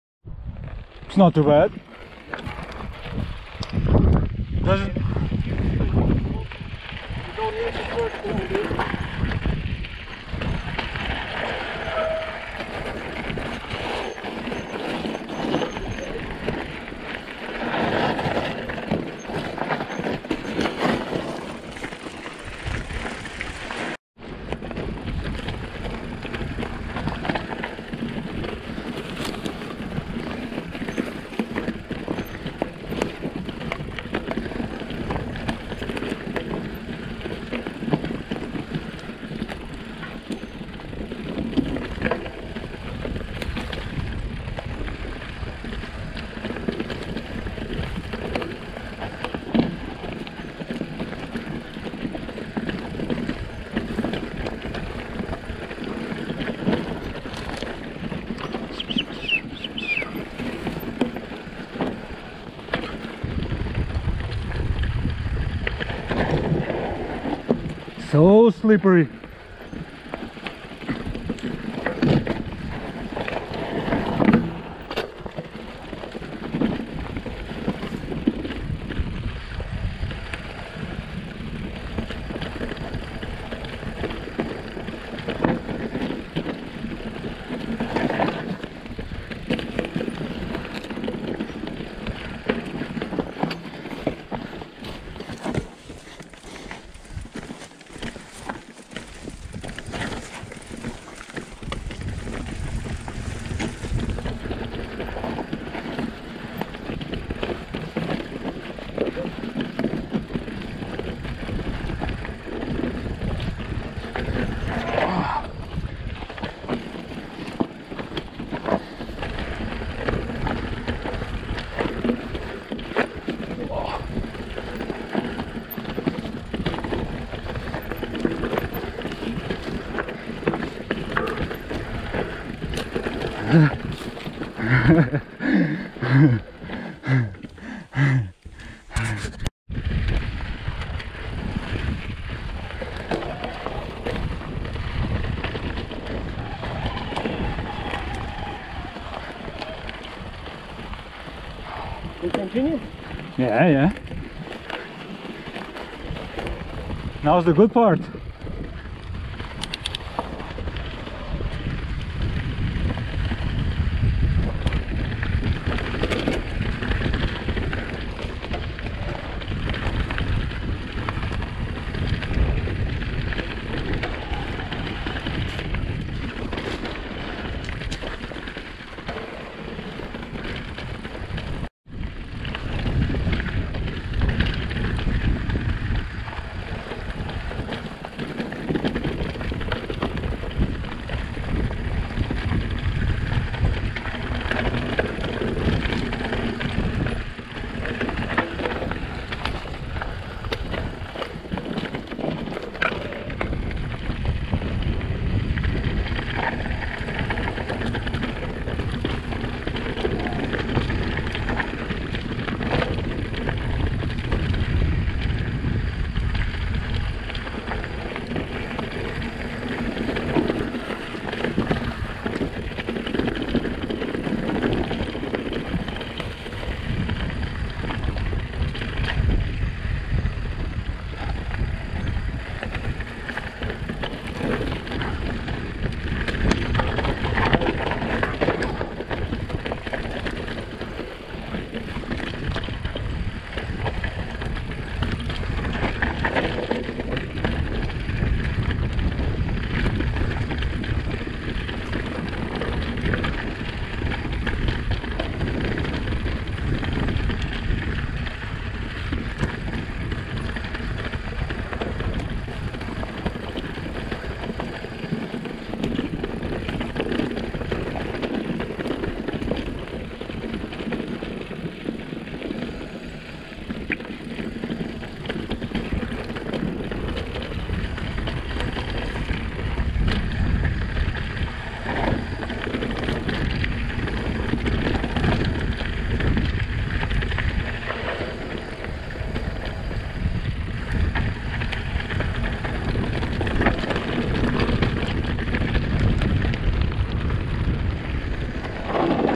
Canary Pine Trail - Tenerife
Descent close to the Teide. Volcanic rock and pine needles.
Pt2